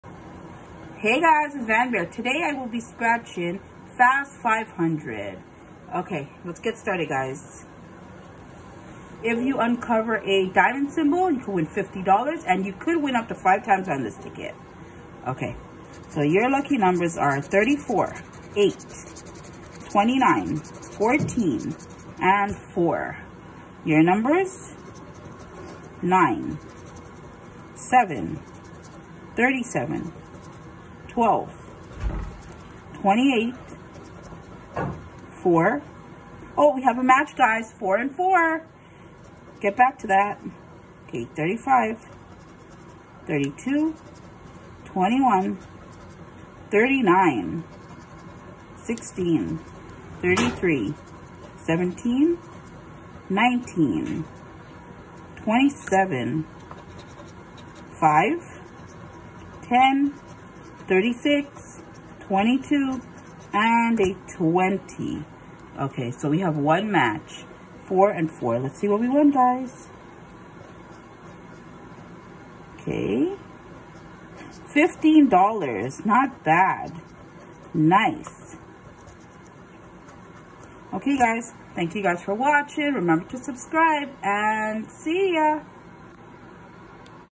NEW!!! FAST $500 OLG SCRATCH TICKET ☘️WINNER🍀
INSTANT Lottery Game No. 2183

Fast $500 2183 tickets
$5 per play.
Prize payout 67.01 per cent.
Overall odds of winning: 1 in 4.56.
$500 top prize.

Uncover a “DIAMOND” symbol in YOUR NUMBERS, win $50.

You could win up to 5 times on this ticket